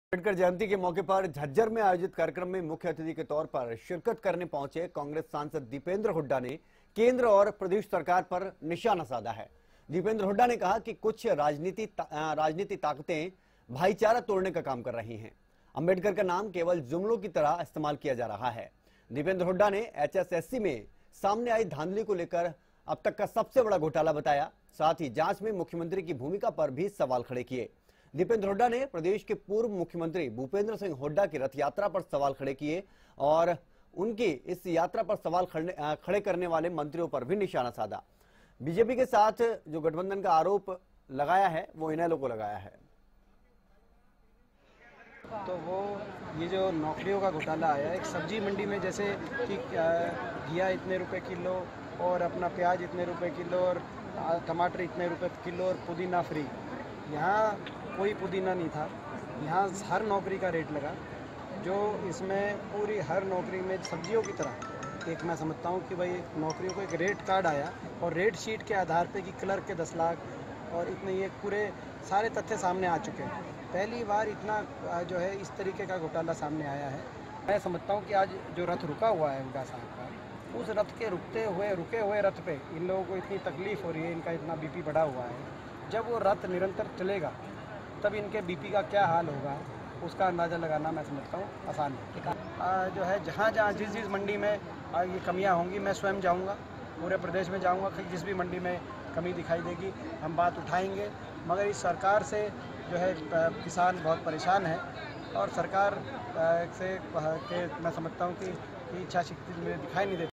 केंद्र और प्रदेश सरकार पर दीपेंद्र हुड्डा का हमला (16/04/2018)
'भाईचारे को तोड़कर राजनीतिक रोटियां सेंक रही कुछ ताकतें’